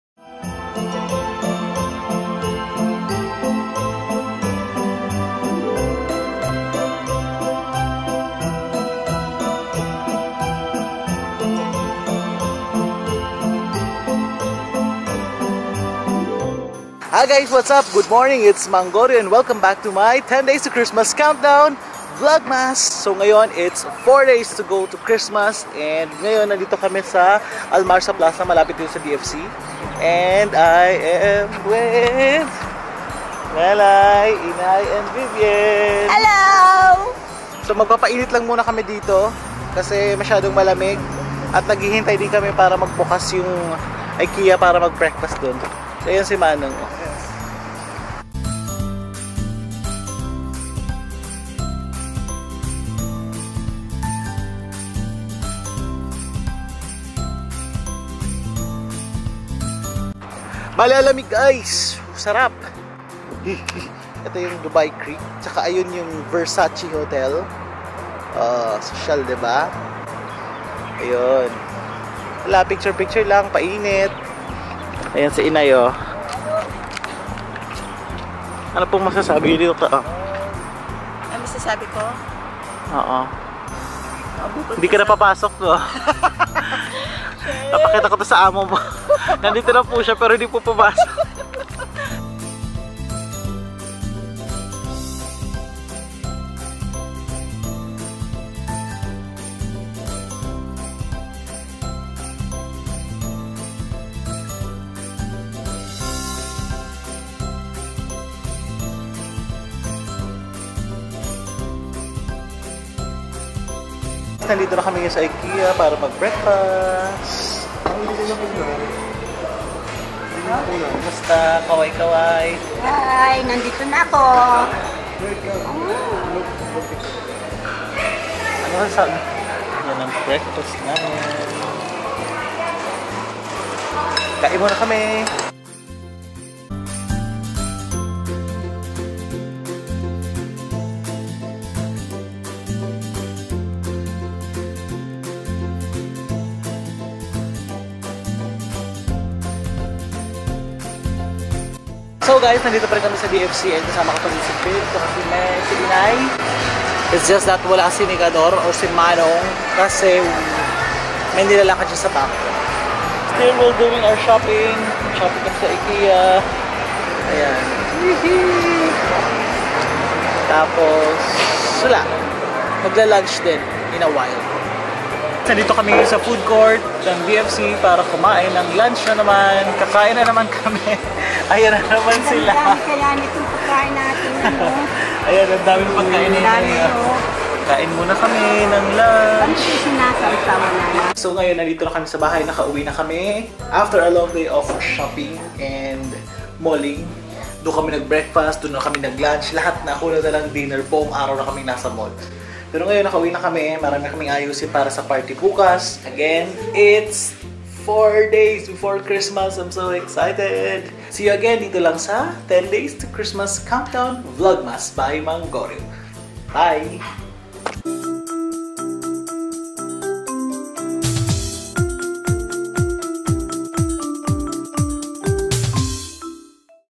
VLOGMAS: Nilamig at nagpa-init, Maghapon sa Mall!
4 days to go before Christmas!
Pasyal pa more! Pa-init pa more!
Ayun na nga nag mga ginawa namin last Dec. 21. :-)